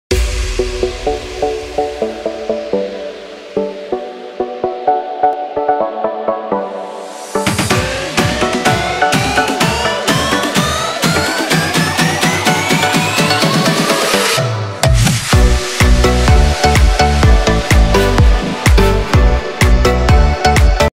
How to detect opioids?
In this video, joyresearch.org demonstrates how the proprietary technology - Drug RevealKit can be used for detecting the presence of opioids and psychedelics. The test is a simple wipe test.